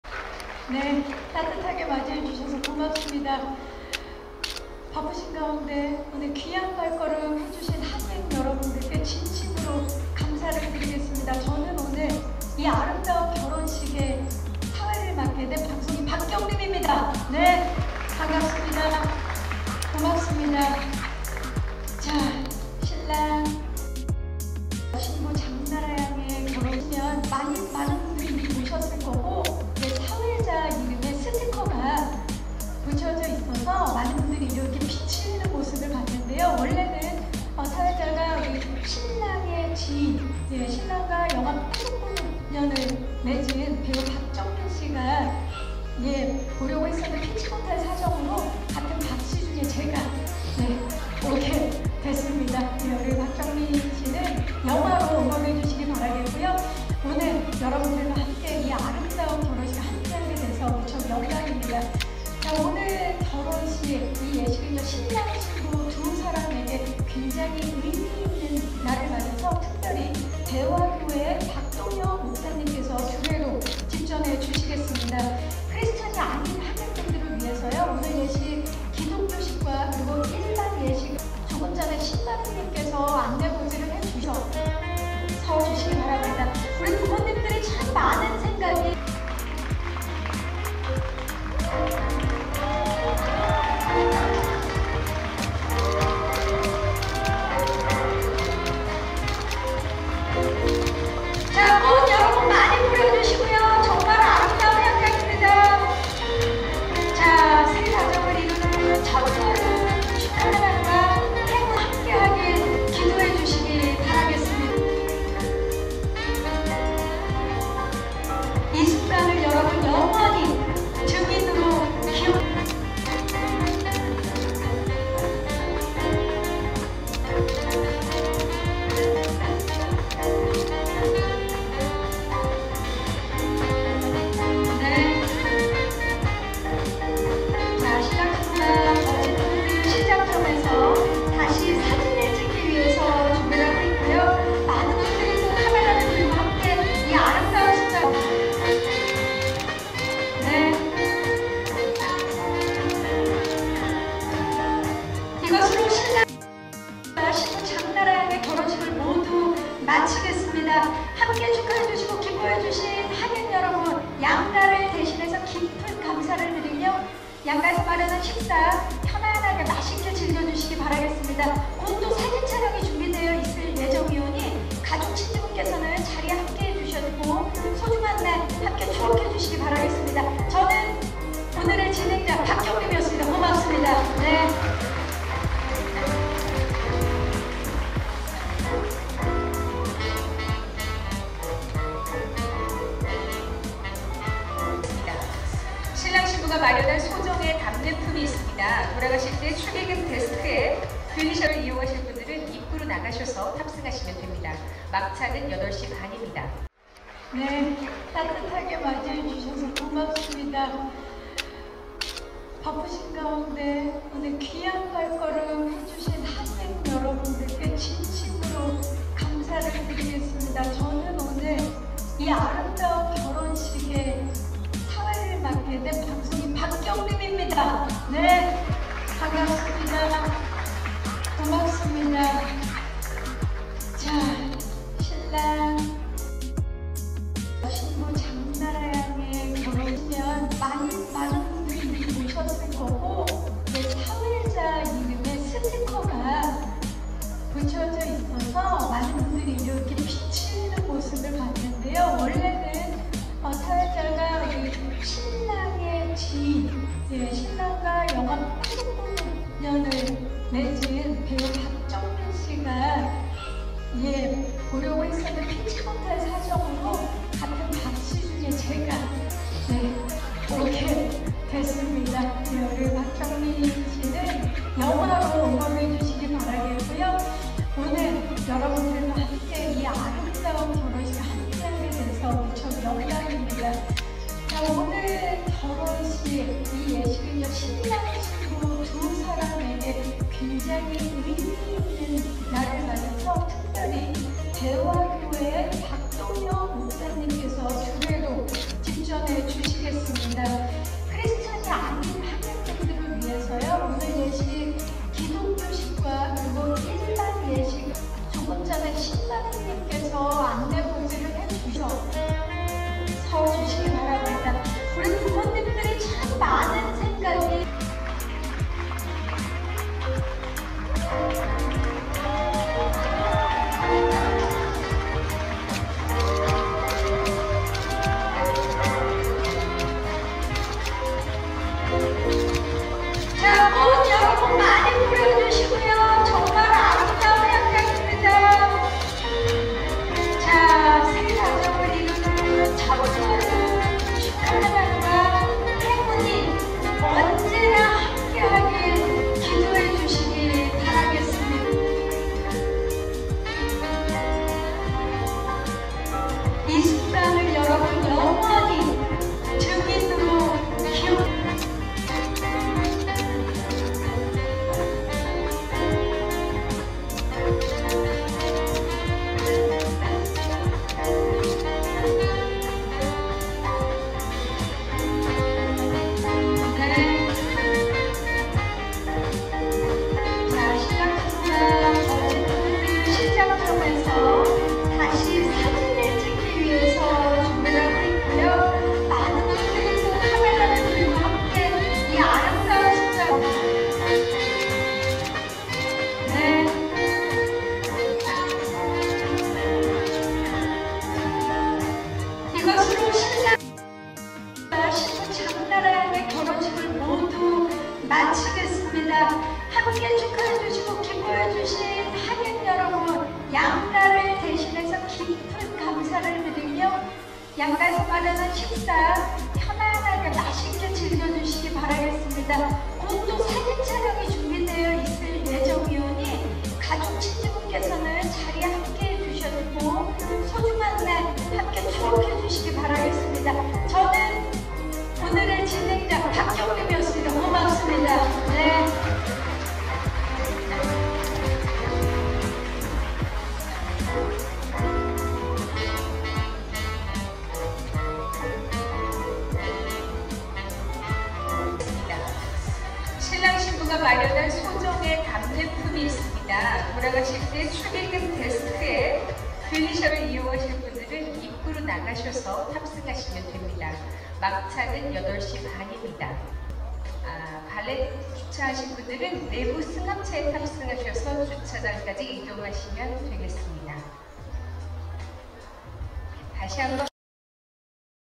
[단독] 장나라♥6살 연하 촬영감독 결혼식 현장 포착
배우 장나라가 26일 서울 모처에서 6세 연하의 촬영감독과 결혼식을 하고 있다.

장나라의 남편은 과거 한 작품에서 인연을 맺은 훈남 촬영감독이다. 두 사람은 약 2년 가까이 교제하다가 지난해 상견례를 마친 뒤 결혼식을 올리기로 했다.